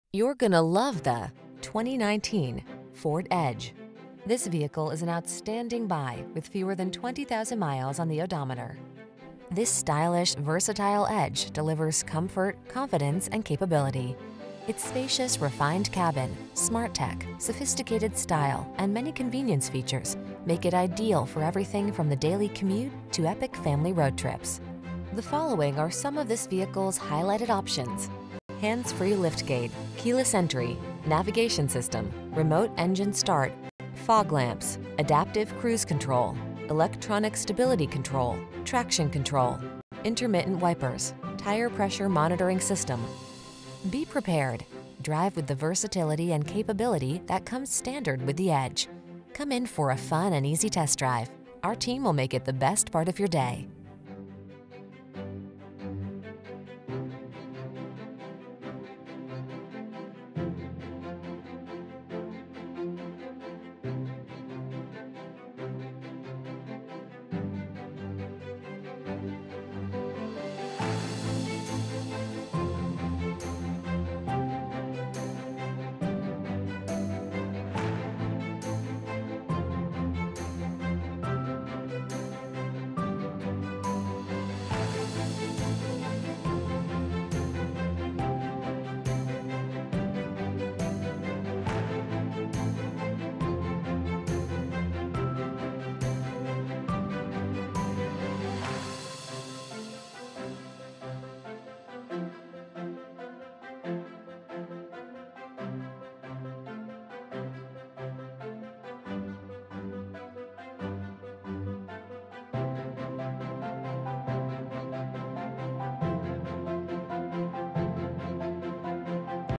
2019 Ford Edge Saline, Ann Arbor, Ypsilanti, Belleville, Milan U04073
Magnetic Used 2019 Ford Edge available in 7070 East Michigan Avenue, Saline, Michigan at Briarwood Ford. Servicing the Saline, Ann Arbor, Ypsilanti, Belleville, Milan, MI area.

2019 Ford Edge SEL - Stock#: U04073 - VIN#: 2FMPK4J94KBC19215

Briarwood Ford
7070 East Michigan Ave